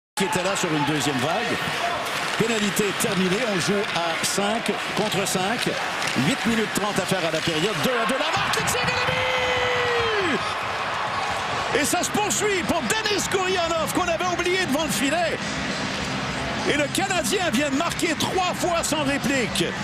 Gurianov Scores his 3rd Goal in 3 Straight Games
Denis Gurianov scores his 3rd goal in his last 3 straight games